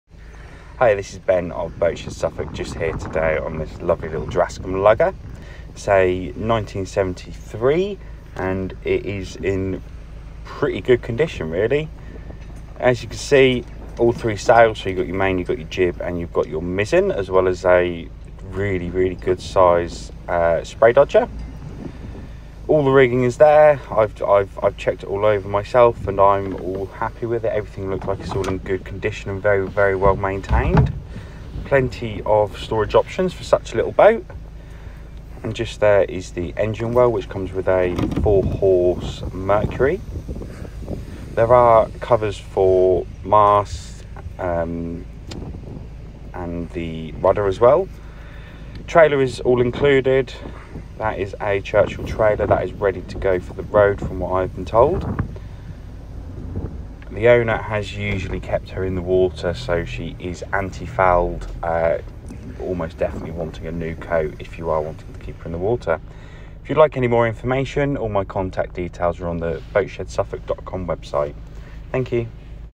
Drascombe Lugger - Boatshed - Boat Ref#311742
Drascombe Lugger for sale with Boatshed Suffolk Photos and video taken by Boatshed Suffolk